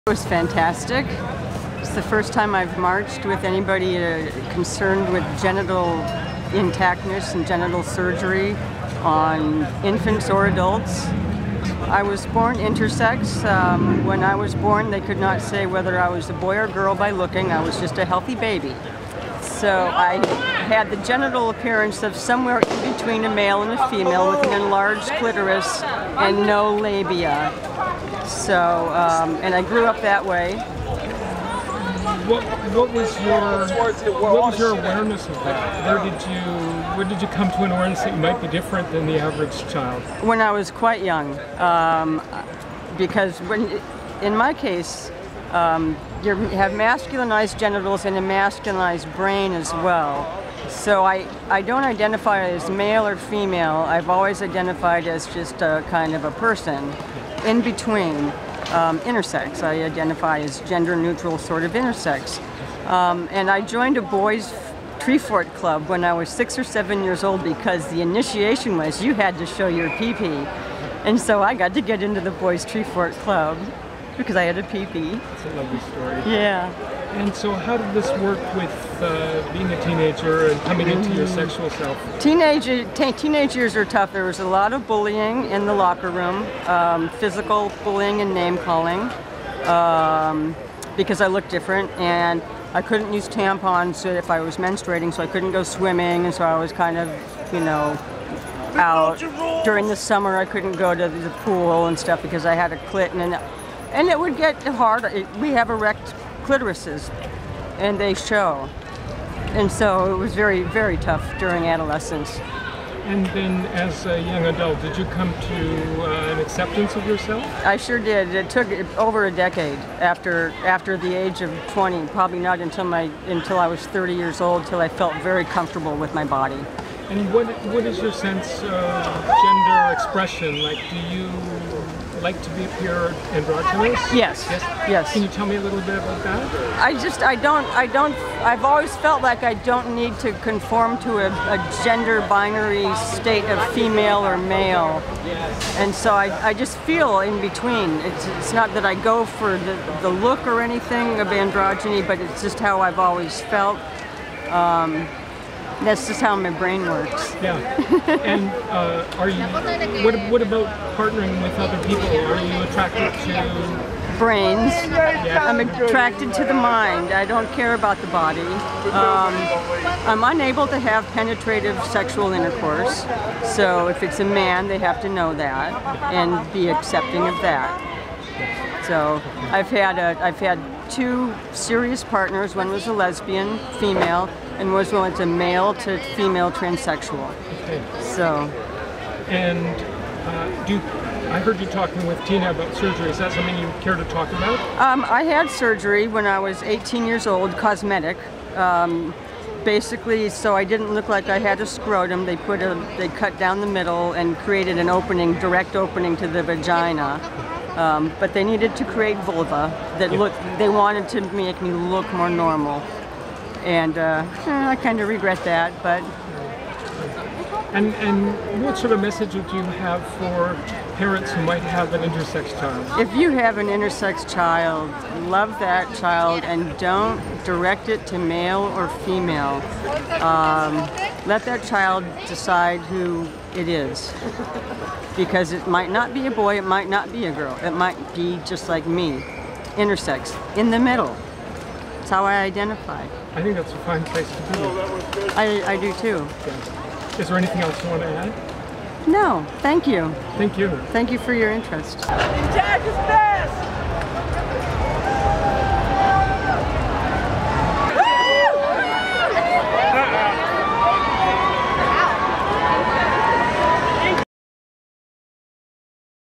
Intersex - In the Middle
After marching with the Bay Area Intactivists in the SF Pride Parade, Charlie Suzi Chapler discusses their personal experience with intersex.